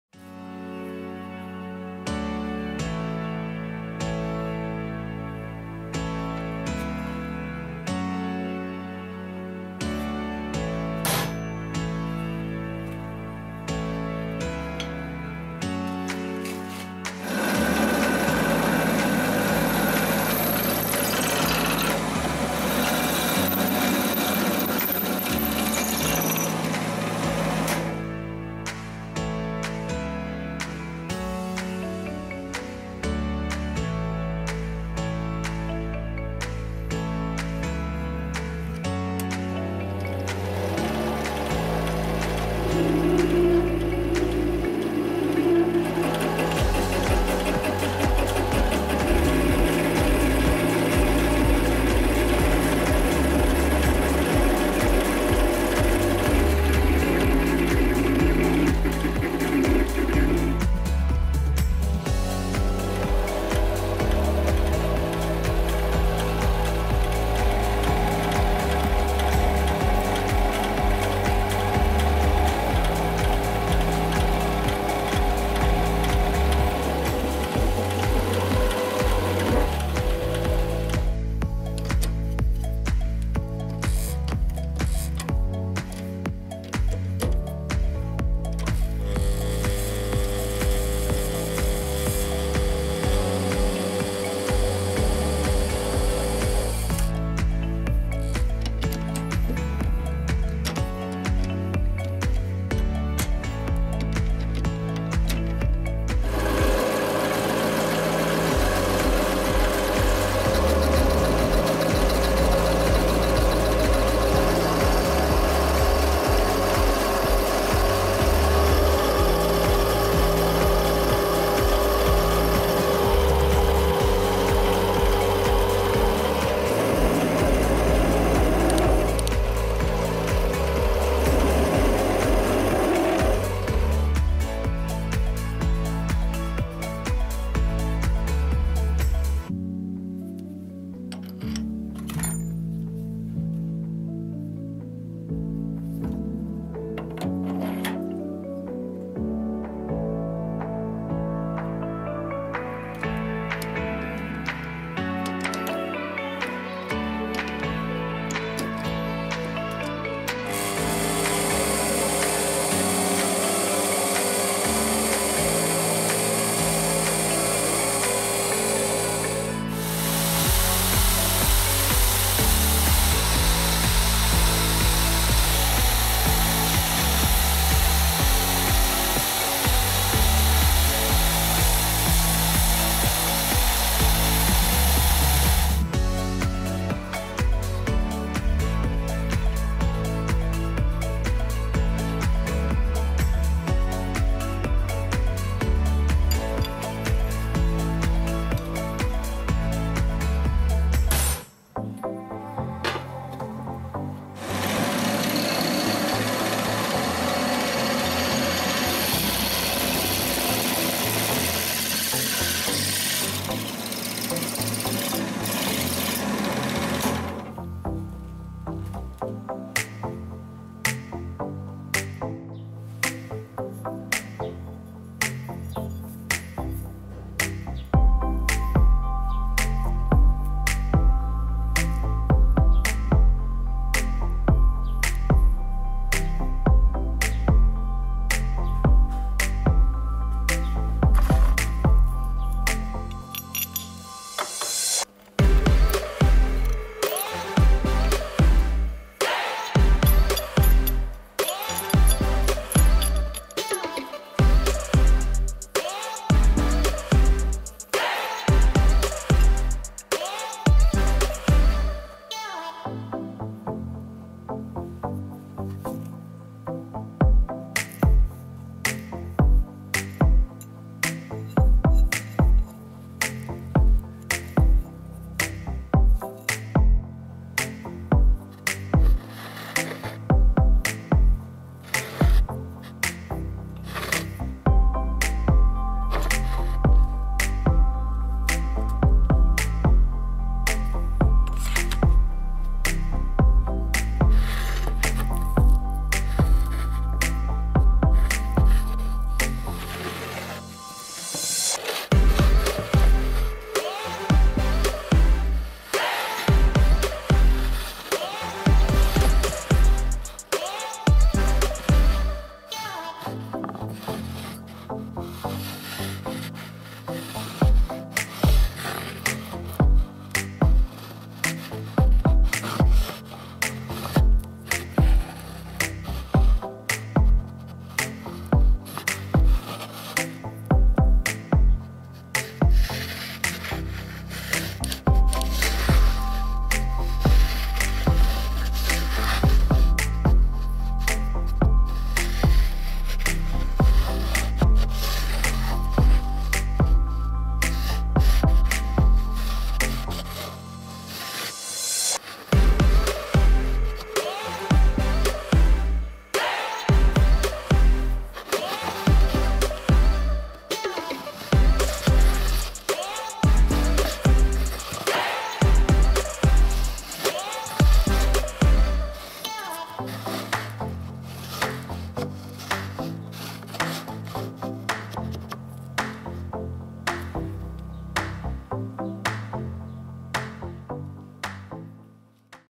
A very useful marking tool Simple barbell
Hallo friends!
In this video I will show how I was making this tool for marking. This is such a cute and simple barbell.